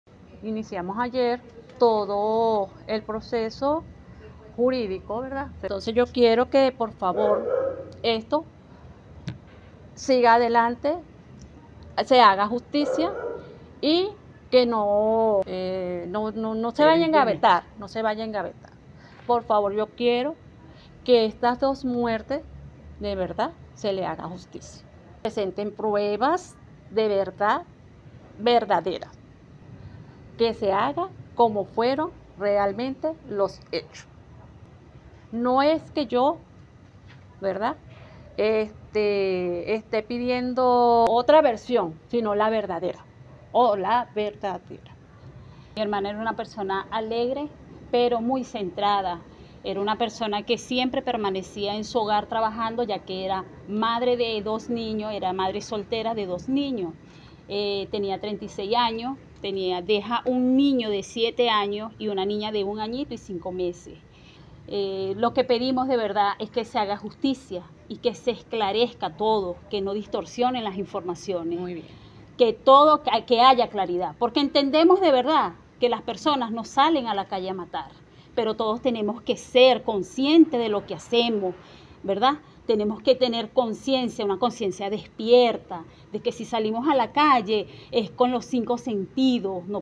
Detenido presunto responsable del accidente donde murió periodista larense y su acompañante
El presunto responsable del accidente de tránsito donde perdieron la vida dos jóvenes larenses, Yorvid García y Yuly Castillo, el día sábado 10 de agosto en la Intercomunal Barquisimeto – Acarigua; fue presentado el día de ayer 12 de agosto, y se encuentra detenido y bajo investigación.

Deneiker Soñarte de 23 años, conductor del vehículo marca Chevrolet modelo Tahoe, quien al parecer manejaba a exceso de velocidad y bajo los efectos del alcohol; se desempeñaba como funcionario de La Guardia del Pueblo.

A través de una rueda de prensa, Milexa García, madre de Yorvid, y Yelitza Castillo, hermana de Yuly, pidieron encarecidamente que se haga justicia por la muerte de sus familiares.

García hizo énfasis en que el caso sea investigado exhaustivamente y que no sea pasado por alto ningún detalle, «queremos que se presenten las pruebas verdaderas, que se investigue como realmente fueron los hechos».